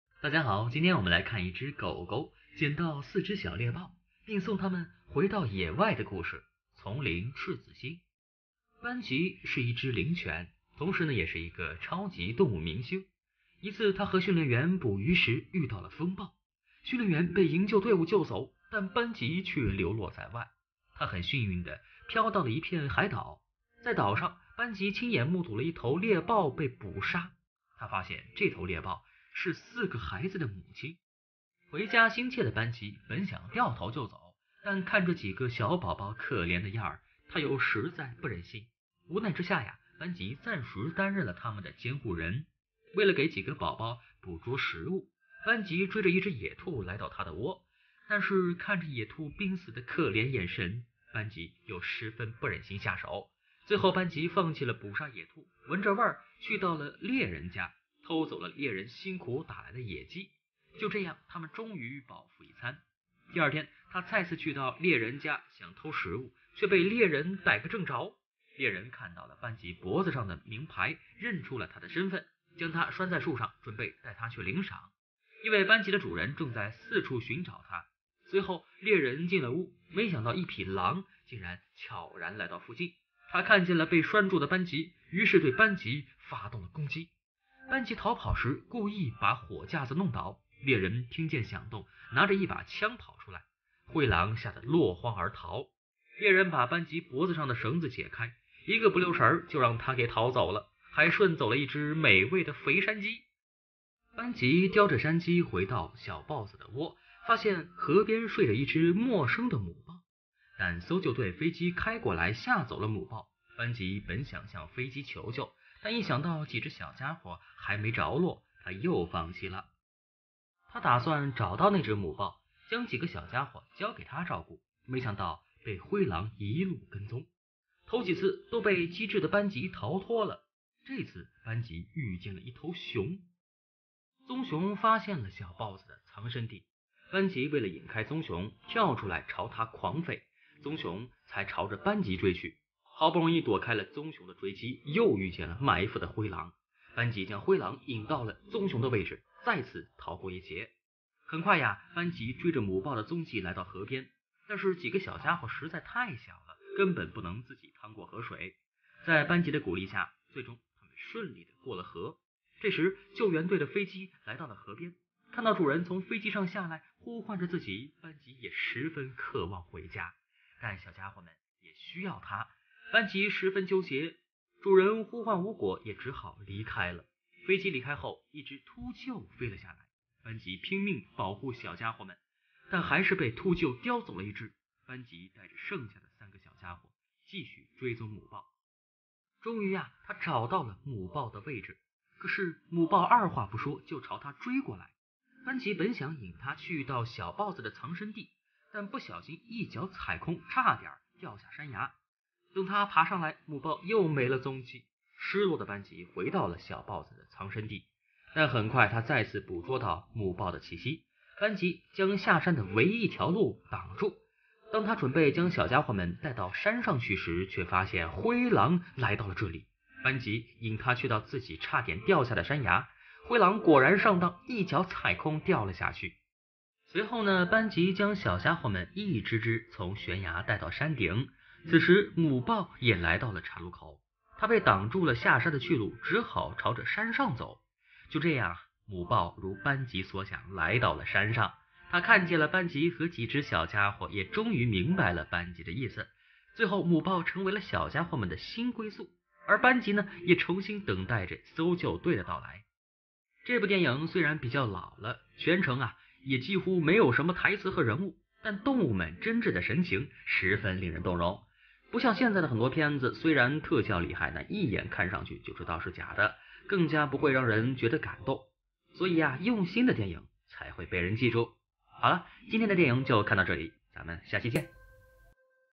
狗狗捡到四只小猎豹，一路护送它们回野外，并为它们找到一个新家
电影、影视、冒险、野外、剧情